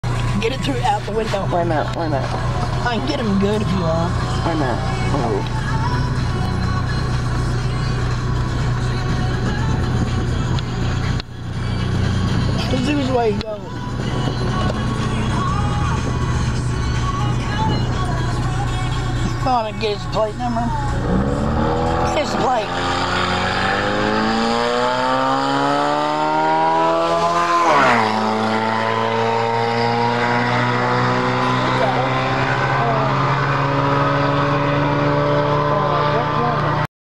Ripping up the fields in Transylvania
This guy had fun ripping up the playing fields in Transylvania County NC. When the police want to know who did it...they know who it was.